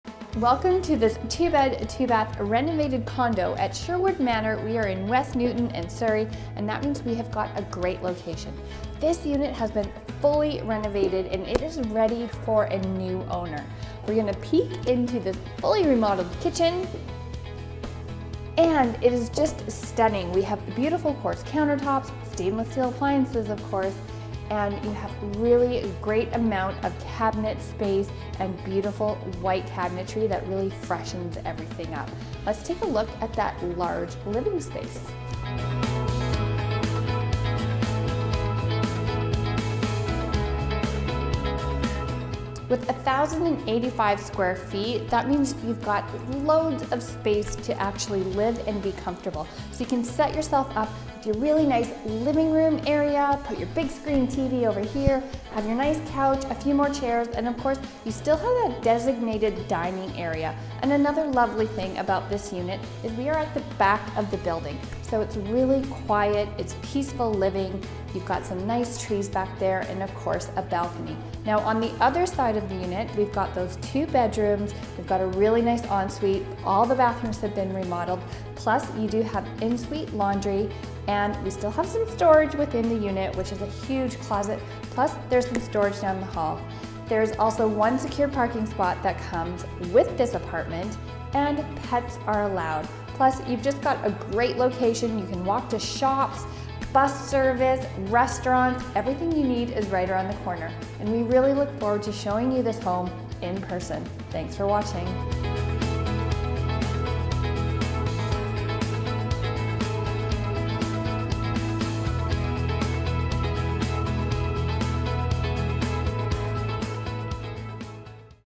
SOLD! | 205 7175 134th Street Surrey BC | MLS R2740140
Fully renovated & ready to move in! New flooring, kitchen cabinets, quartz countertops throughout, backsplash, stainless steel appliances, fresh paint, plus 2 new full bathrooms. Just packup and move in!

Centrally located in West Newton, enjoy easy access to shopping, restaurants, schools, amenities & Unwin Park. Located at the back of the building means quiet living to enjoy unwinding after a long day. Oversized living room, separate dining room & stunning white kitchen which flow nicely together. In suite storage, in suite laundry plus extra storage space on the same floor making it easy to get your seasonal items.

Quick possession possible, 1 secured underground parking spot, tons of visitor parking, rentals & pets allowed. Call today to book your showing!

Asking $569,900
2 beds | 2 baths
1,085 sq ft
Pets allowed | Rentals allowed